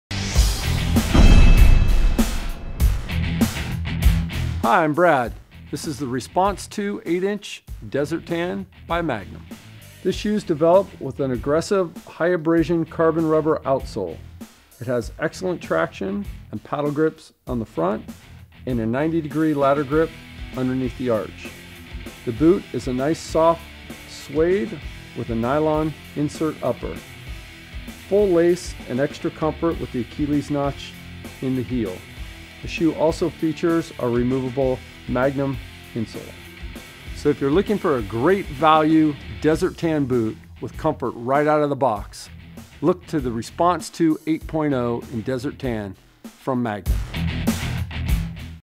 Response II 8 inch Desert Tan - Premium Mens Tactical Boot from Magnum (5470)
Leather / 900 denier nylon upper
Suede collar
Bluecher lacing system
Moisture-wicking lining
Steel shank
Comfort contoured removable sockliner
High traction, slip-resistant outsole